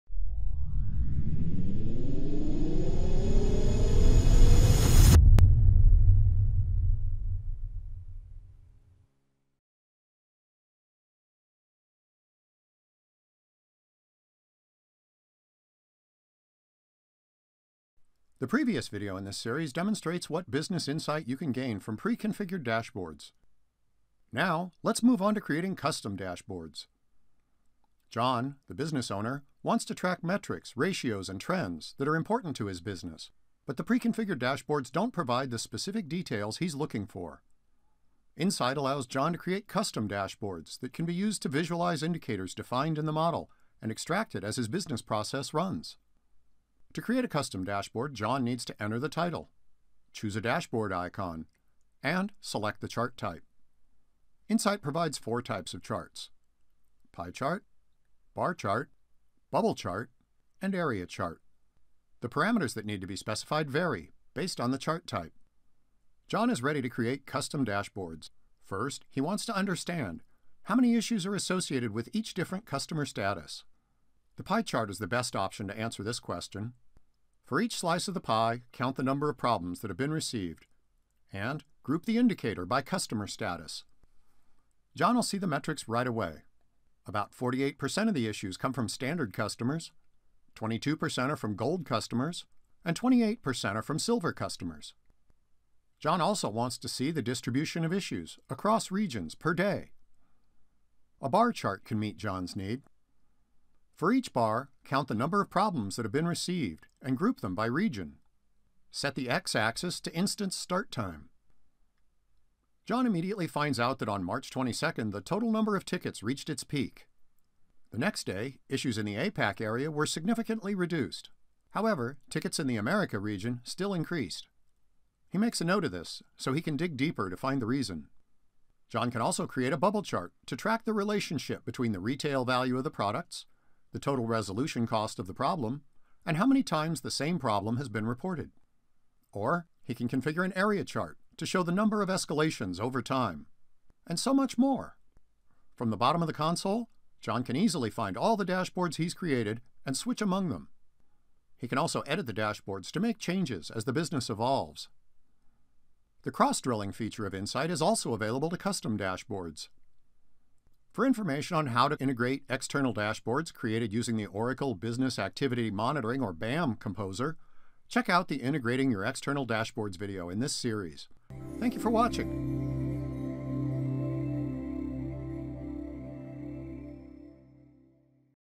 4 – Oracle Real-Time Integration Business Insight: Custom Dashboards
This short video will be part of the Introducing Oracle Real-Time Integration Business Insight (aka Insight) video series. This video demonstrates how to create and use custom dashboards to get more specific insight into your business.